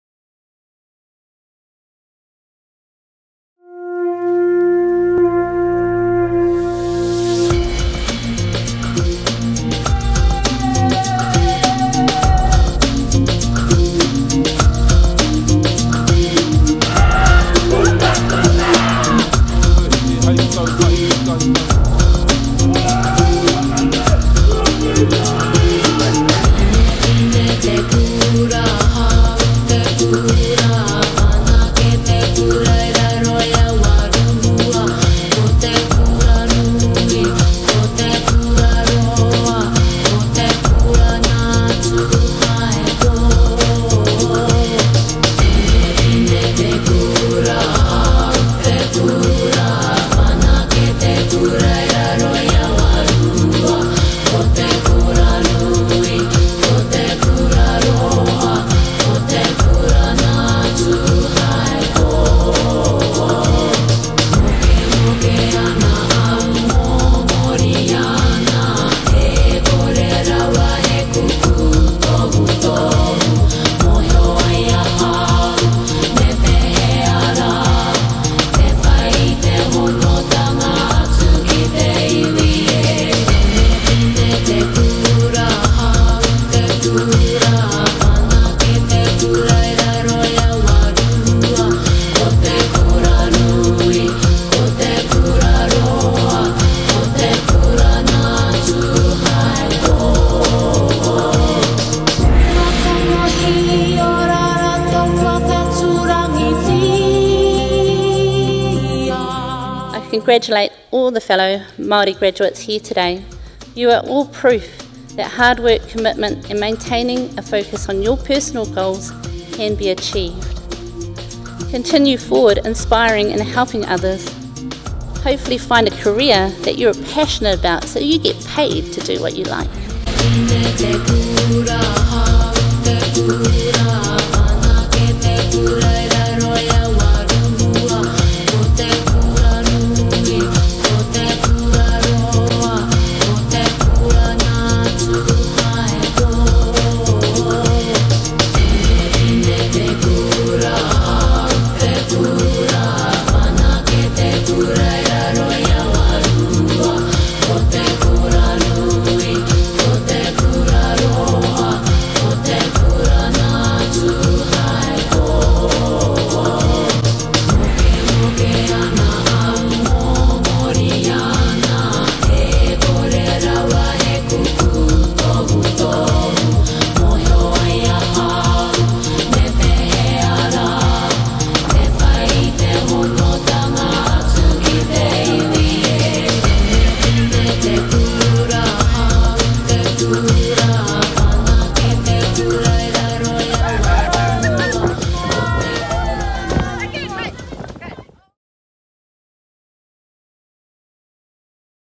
Highlights of the Celebration to Honour Māori Graduates, Manawatū 2019
Massey University acknowledges its Māori graduates with a special Celebration to Honour Māori Graduates. This ceremony was held on May 16th, at the Regent on Broadway in Palmerston North.